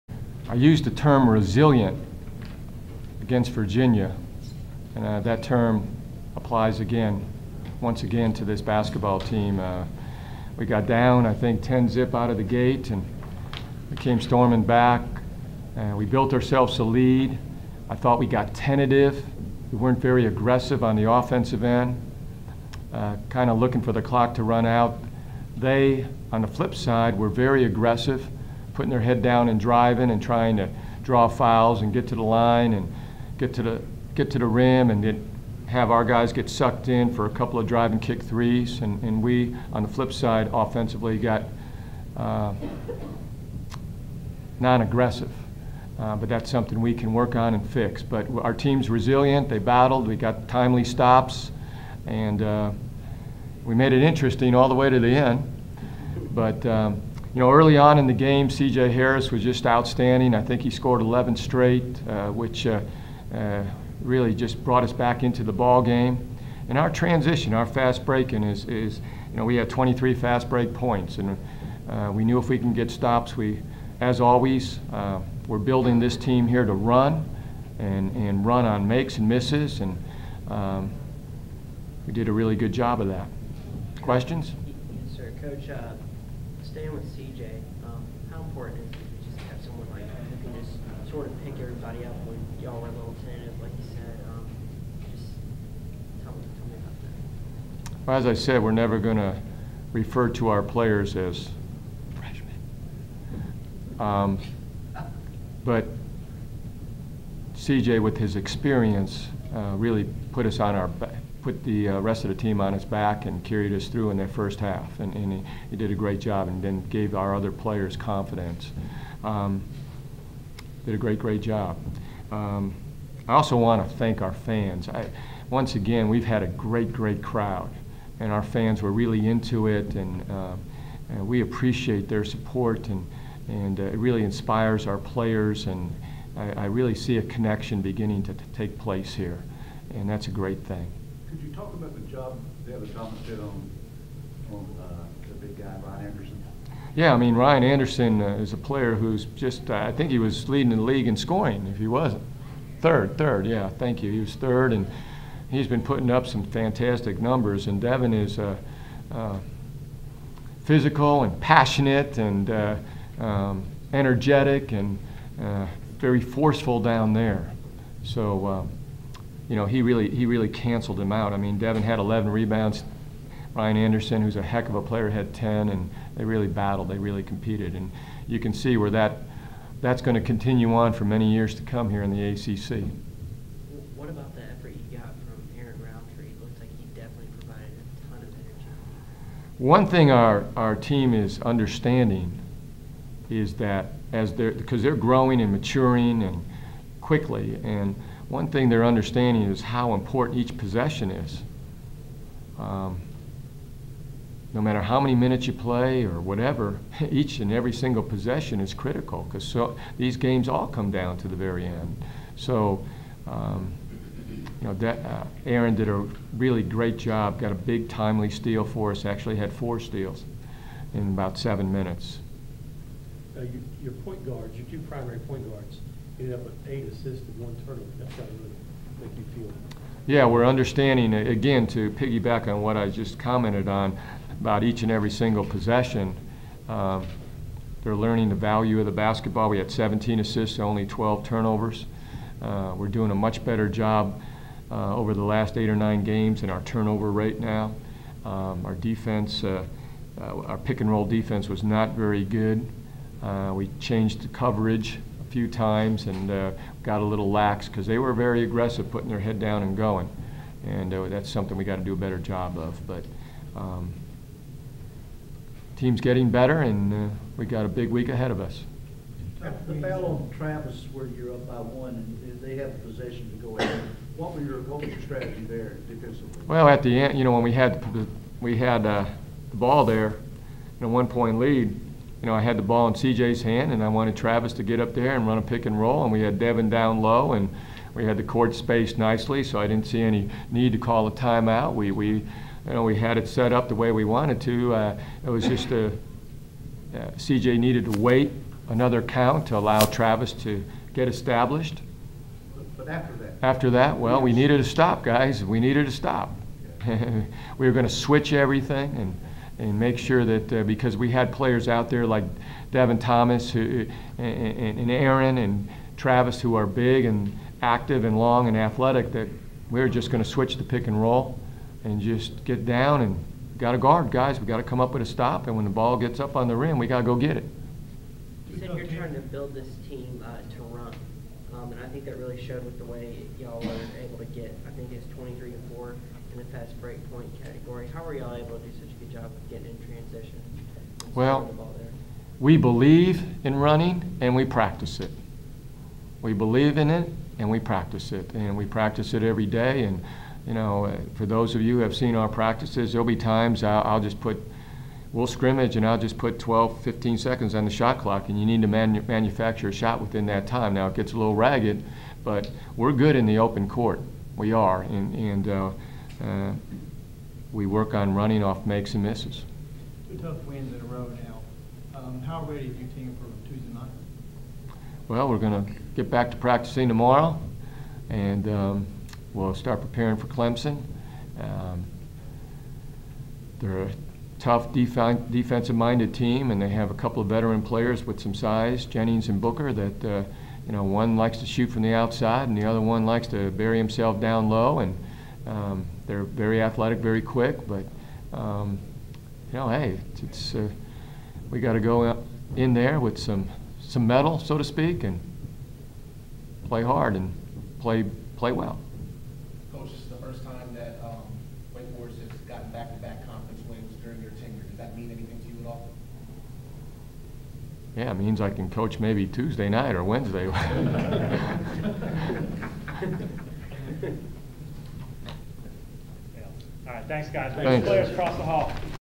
Jeff Bzdelik Postgame Press Conference - vs. Boston College
Wake Forest head coach Jeff Bzdelik talked to the media following the Demon Deacons' 75-72 win over Boston College on Jan. 12, 2013.